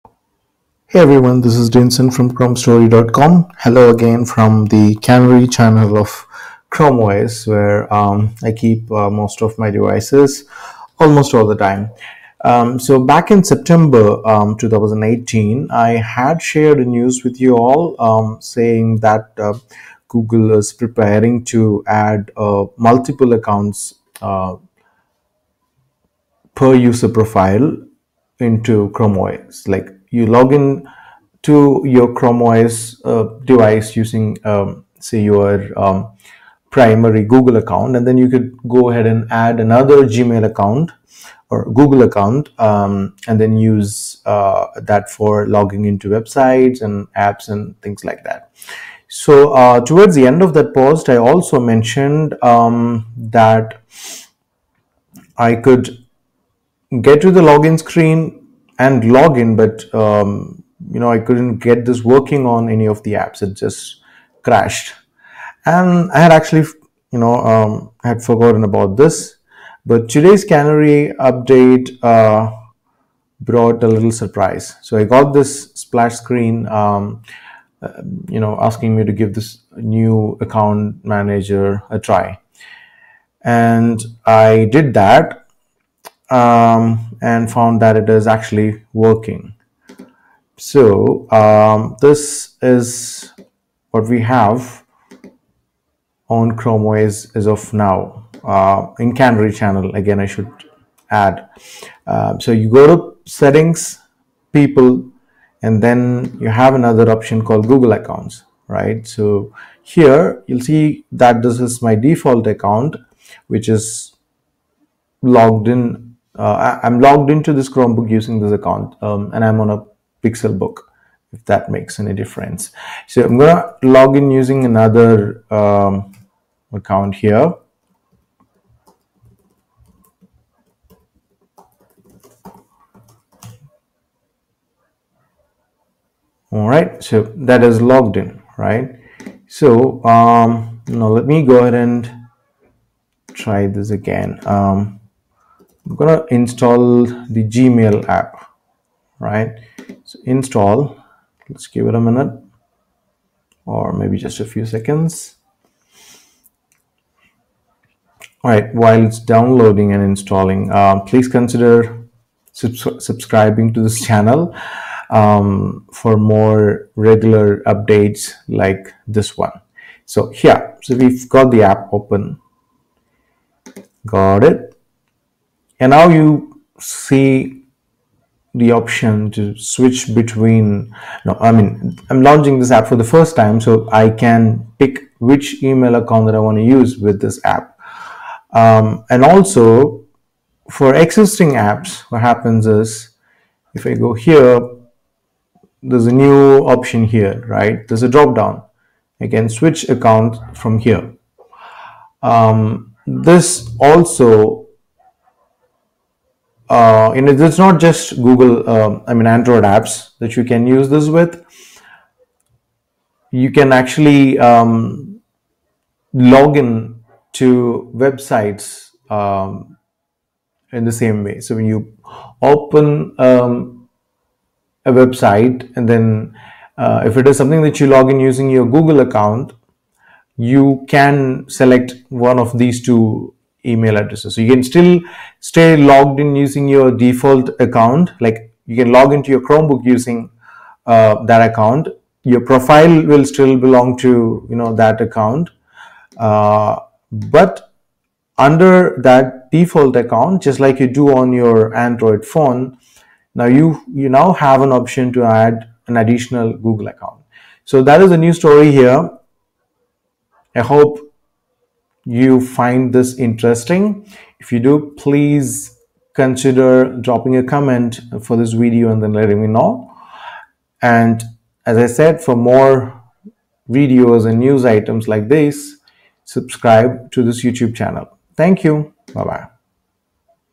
Meet the New Account Manager for Chrome OS - Login to Multiple Google Accounts
This new Chrome OS feature allows you to login to multiple Google accounts from the same profile. You could use Android apps with other Google accounts and also switch between these apps.